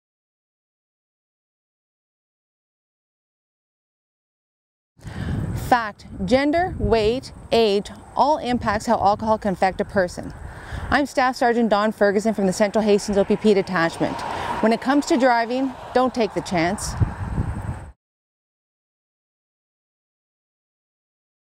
Home Safe for the Holidays - Myth #1
Video #1 from the HomeSafeForTheHolidays campaign. This campaign is a collaboration between Hastings Prince Edward Public Health and the Quinte Region Traffic Coalition. Each video aims to dispel myths about impaired driving and educate about the importance of planning for a safe ride home.